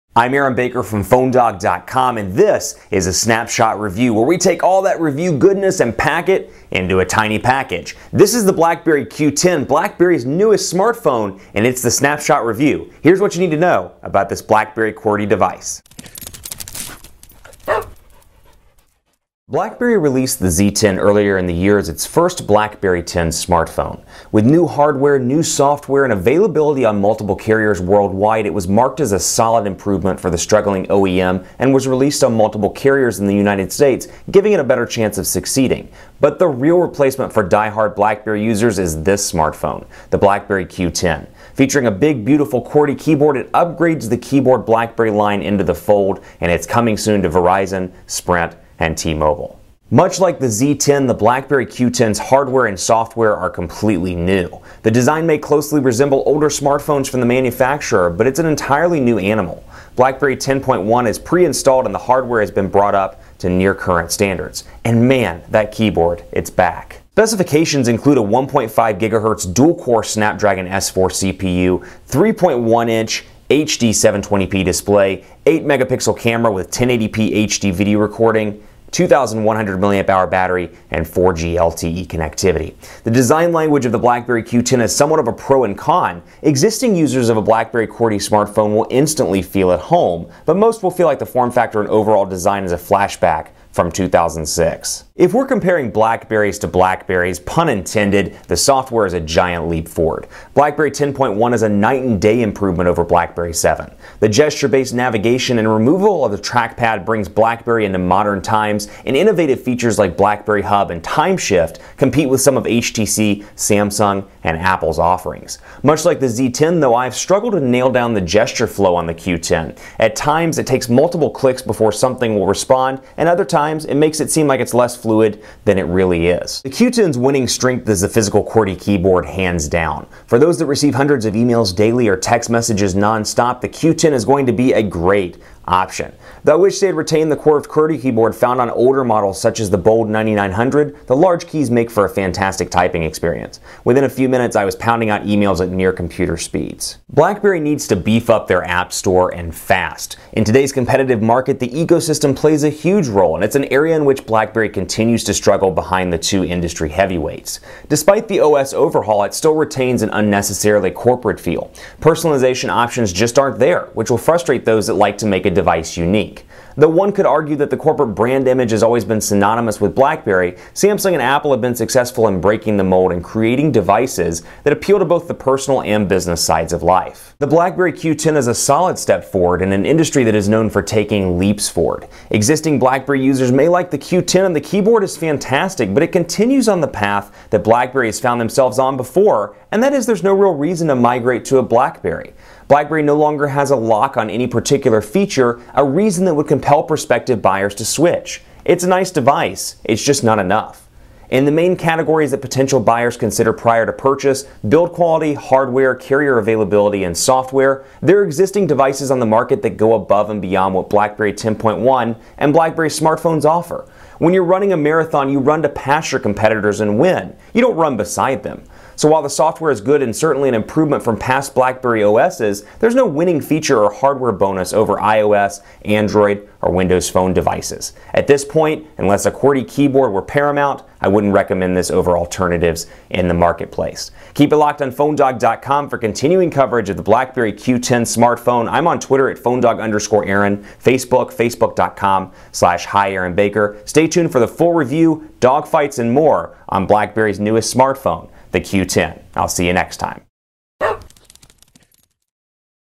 BlackBerry Q10 Snapshot Review
BlackBerry released the Z10 earlier in the year as its first BlackBerry 10 smartphone.  With new hardware, new software, and availability on multiple carriers worldwide, it was marked as a solid improvement for the struggling OEM and was released on multiple carriers in the United States, giving it a better chance of succeeding.

But the real replacement for die-hard BlackBerry users is this smartphone: the BlackBerry Q10.  Featuring a big, beautiful physical QWERTY keyboard, it upgrades the keyboard BlackBerry line into the fold.  It's coming soon to Verizon Wireless, Sprint, and T-Mobile.